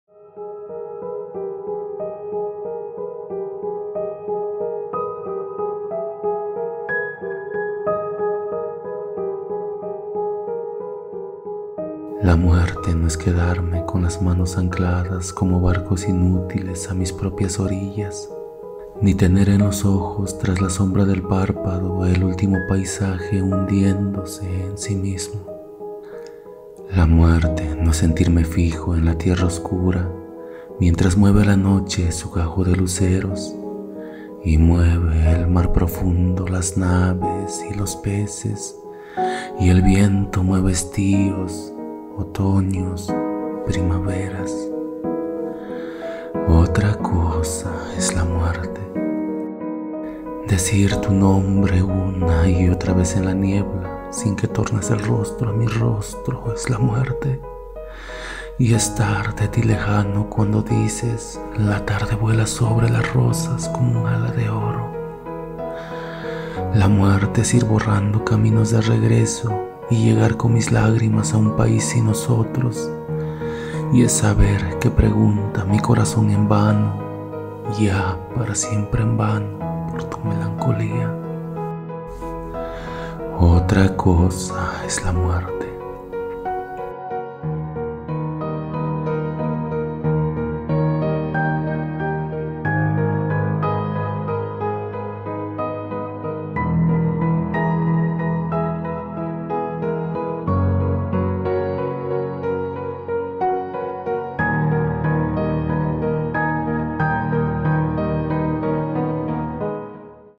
😭(OTRA COSA ES LA MUERTE) Meira Delmar - Muerte Mía (POEMA LEÍDO, RECITADO)
😢ESTAR DE TI LEJANA ES LA MUERTE😢

Una de las poetas latinoamericanas más importantes del siglo pasado hace presencia en este canal. Con un tono melancólico y armonioso va construyendo la auténtica muerte, la de estarse sola. Disfrútalo en la interpretación original de Michael de Victoria.